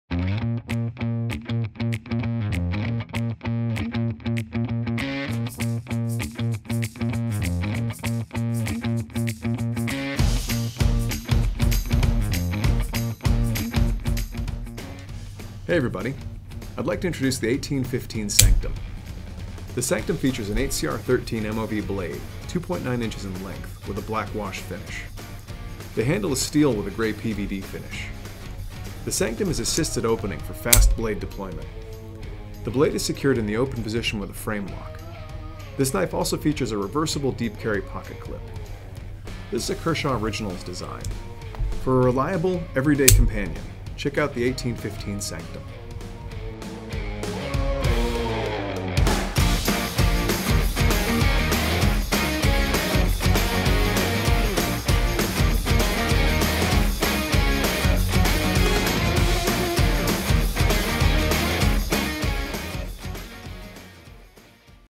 Kershaw Sanctum- Model 1815
We're celebrating 50 years, and we're just getting started!

The Sanctum is stylish and practical, everything you need for everyday carry. Complete work tasks with the pointy BlackWash™ blade, from large slicing cuts to fine detail work. It’s easy to grip the Sanctum using the contoured handle, which is made of reliable stainless steel in a gray PVD finish. This folder opens easily using fast assisted opening, and it locks into place with a sturdy frame lock. Additional features include a reversible deep-carry pocketclip and a handsome custom lanyard hole. It all adds up to an EDC that will appeal to almost anyone.

Steel: 8Cr13MoV
Handle: Stainless Steel
Blade: 2.98 in. (7.6 cm)
Overall: 7.2 in. (18.3 cm)
Weight: 3.7 oz. (105 g)

Tag us in your knife videos.